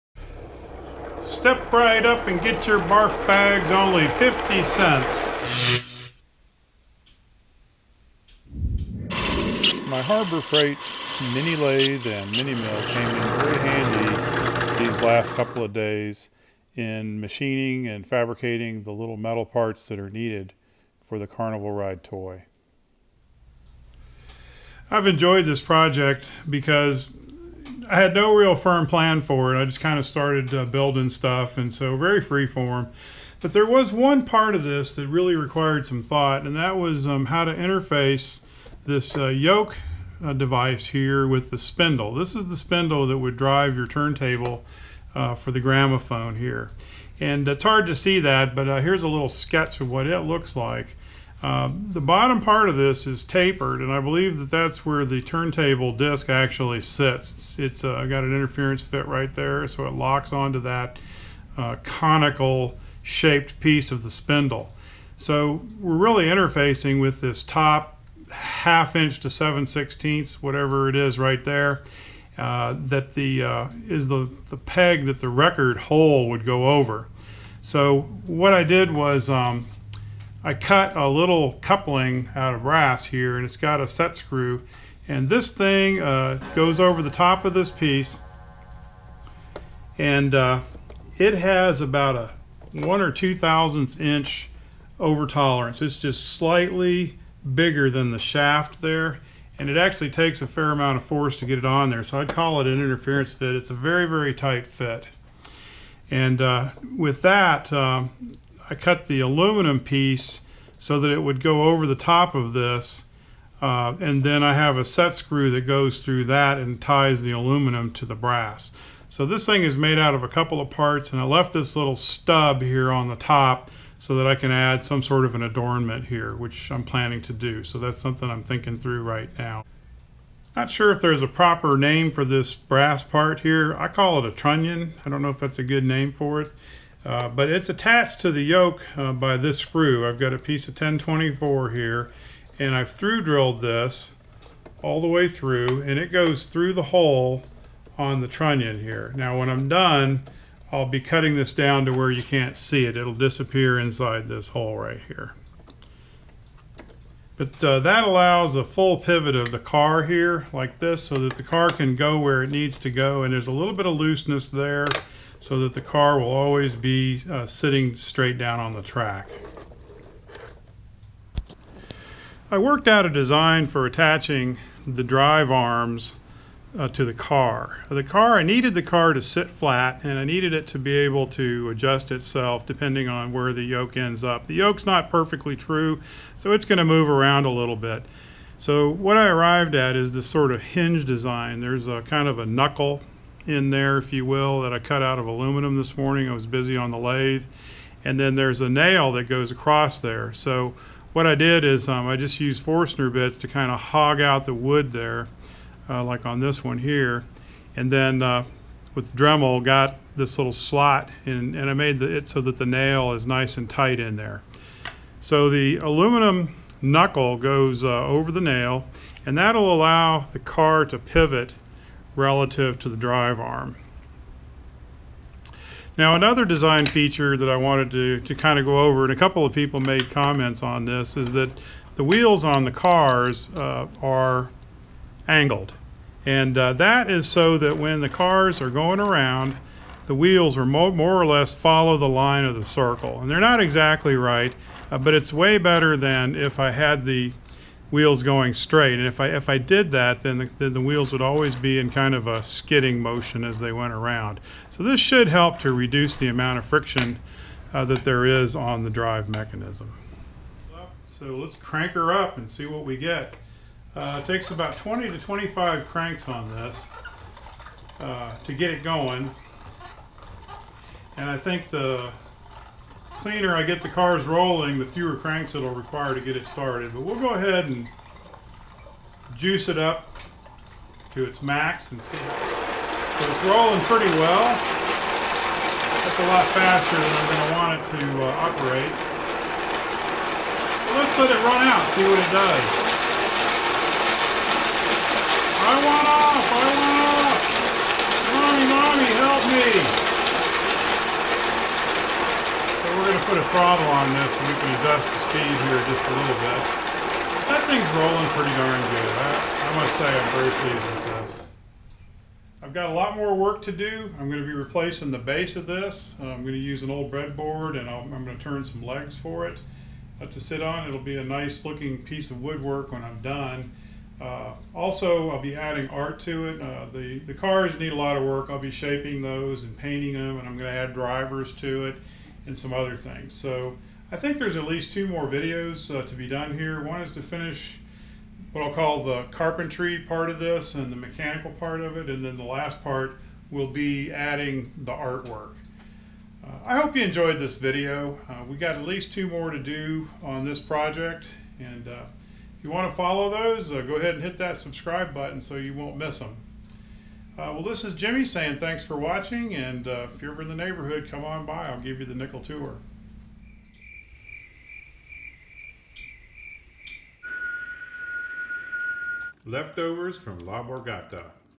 Making a Model Carnival Kiddie Ride Toy Using a Gramophone Spring Motor -Part II
The basic elements are roughed out and it operates well. Need to replace the wood elements and add art to make it look like a toy. Be sure to watch Part I if you haven't already seen it.  I expect two more videos will be needed to capture the rest of this project. Here is a link to Part I if you missed it